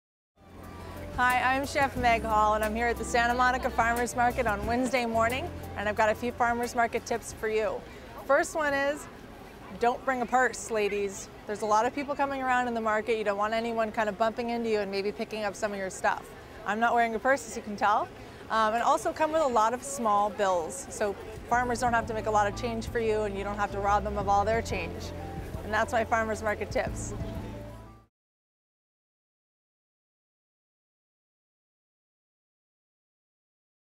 Chef Tips: No Purse
Chef Meg gives tips on surviving the Santa Monica Farmer's Market.